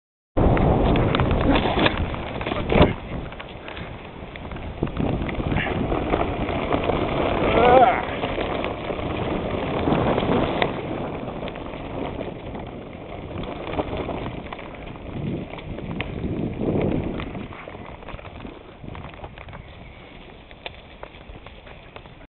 fast corner
me doing just one part, the opening corner, of a good track near where i live. in first person.